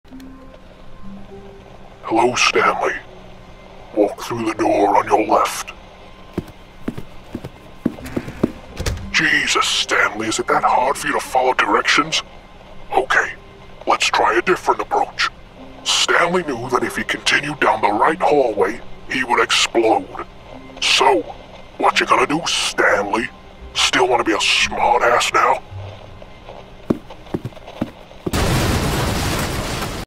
Hello Stanley. Walk through the door on your left
Stanley decided not to go through the door on his left, and try out my brand new 'eight!' button, but instead chose to explode, like the disobedient clod that he was.

Hi, Al. This video is a dub of a Tumblr meme about a puppet, similar to Billy / Jigsaw from Saw, telling Stanley, from The Stanley Parable, to walk through the door on their left. Please show it to people who will like it. Thank you.